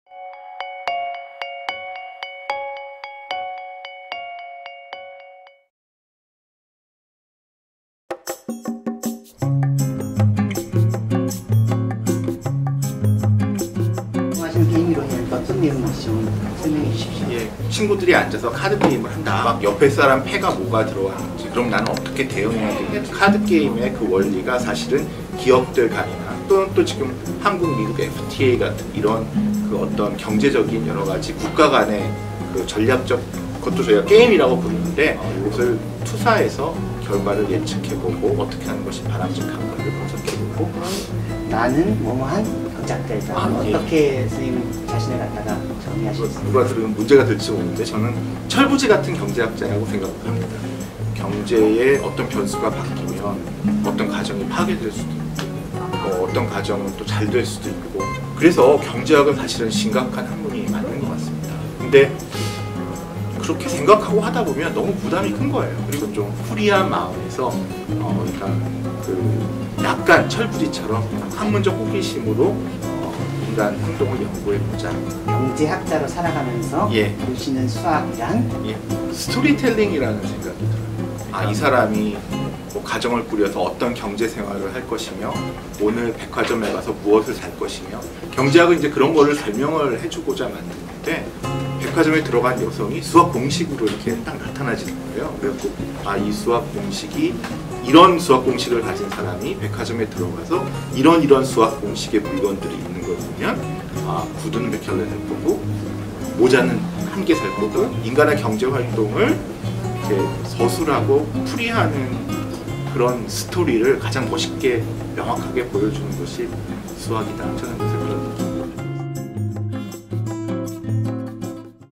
[석학인터뷰] 한순구_ 나는 철부지같은 경제학자다 | 2018 봄 카오스 강연 '모든 것의 수數다'
[석학인터뷰] 나는 철부지같은 경제학자다 | 한순구 연세대 경제학부 교수 | 2018 봄 카오스강연 '모든 것의 수다'

경제학자 한순구 교수_경제학에서 수학은 스토리텔링

석학을 만나는 시간 '카오스 석학 인터뷰'

"경제학에서 수학은 스토리텔링"

한순구(연세대 경제학부 교수)

고등학교 1학년 때 문과와 이과 중 한 가지를 택해야 했는데 어째서 문과로 왔는지 아직 한순구 교수 자신도 설명하지 못한다. 너무 막연하고 이해가 잘 되지 않지만, 수학을 이용해서 표현하면 명확하다는 생각을 가지고 있었기에 문과를 결정한 이후의 진로를 경제학으로 결정한 것에는 아무런 고민이 없었다.

중요한 것은 30년간 경제학을 공부하면서 선택에 후회는 없었다는 것이다. 사람들이 자신의 이익을 위해 최선을 다하고 기업들이 다른 기업과의 경쟁에서 이기기 위해 전략을 구사하는 것을 배우고 나서, 인간뿐 아니라 생물들의 세계에서도 경제학의 논리가 적용될 수 있다는 점에도 흥미를 느끼게 되었다.

대학원생 때 읽은 리처드 도킨스의 “이기적인 유전자”를 가장 감명 깊은 책으로 꼽는 것도 이런 이유이다. 보스톤에서 유학하면서 레드 삭스의 골수 팬이 되었고, 최근 경제학을 야구에 적용하여 성공한 이야기를 다룬 “머니볼”이라는 영화도 아주 좋아한다. 보스톤 레드 삭스의 팬으로서 제일 싫어하는 팀은 당연히 뉴욕 양키즈이다. 각종 역사 서적을 읽으면서 역사적 현상들을 경제학적 측면에서 분석해 보는 것으로 여가 시간을 즐기고 있다.

Q. 게임이론을 전공하셨는데, '게임이론'이란 무엇인가요?

Q. 나는 OO한 경제학자다. 라고 한다면 어떻게 자신을 정의하실 수 있을까요?

Q. 경제학자로 살아가면서 바라보는 수학이란 어떠한 것인가요?